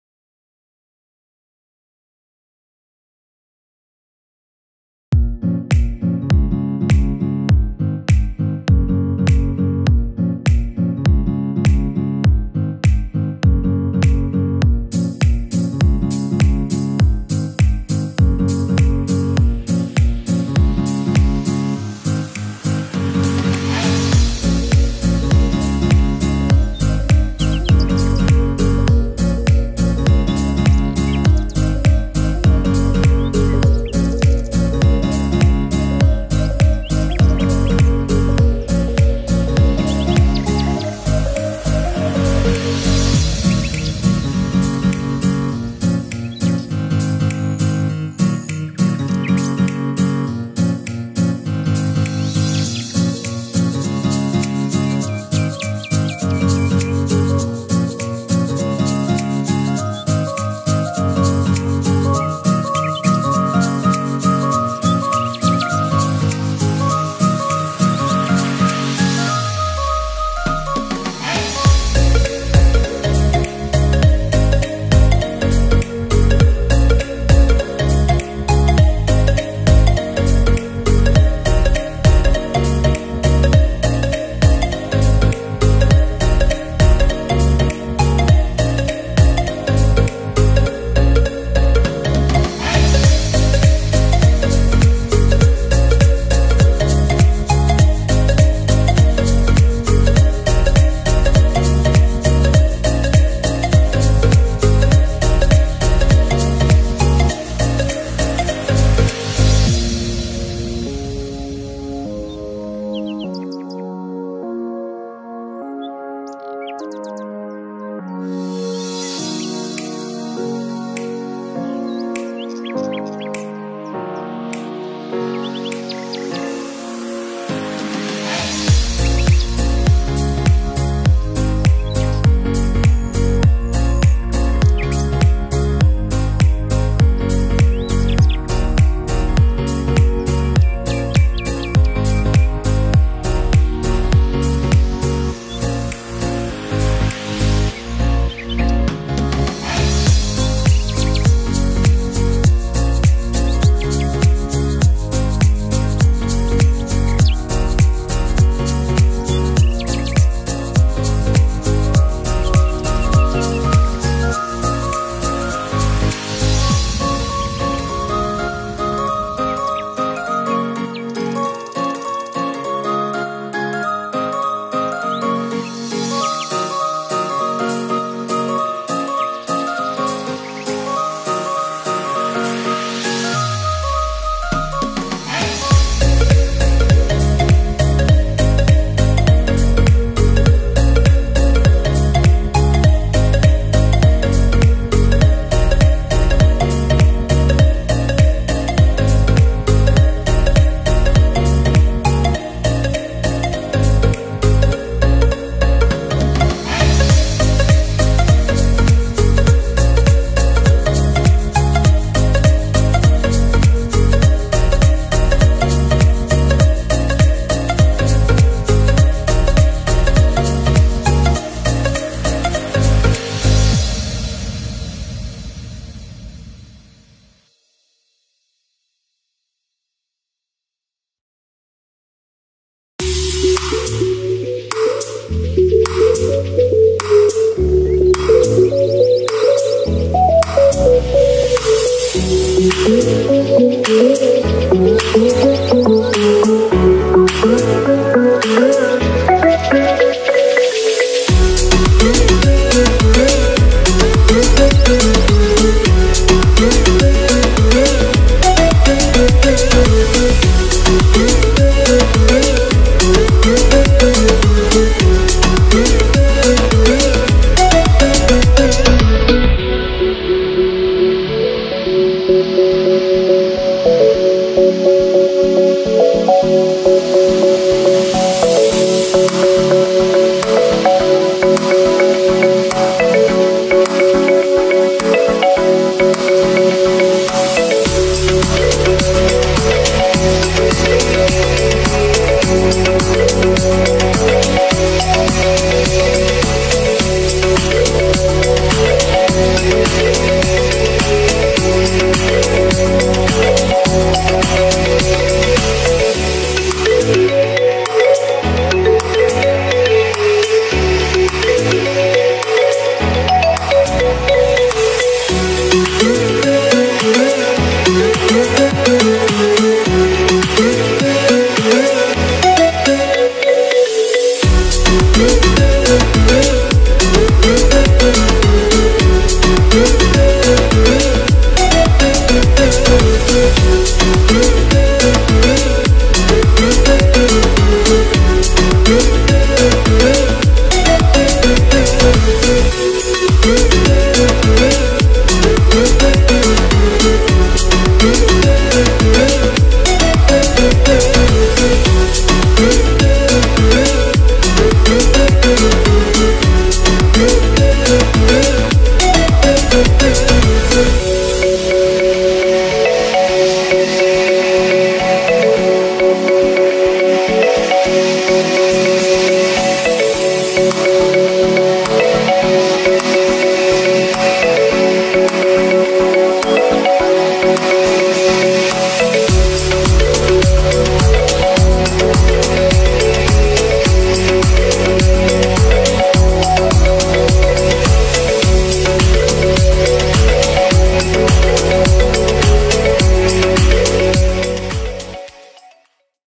Take a hike video process
A video process of the poster inspired by the great art of instagram's @c.kritzelt a nostalgic approach to the mountains and stars.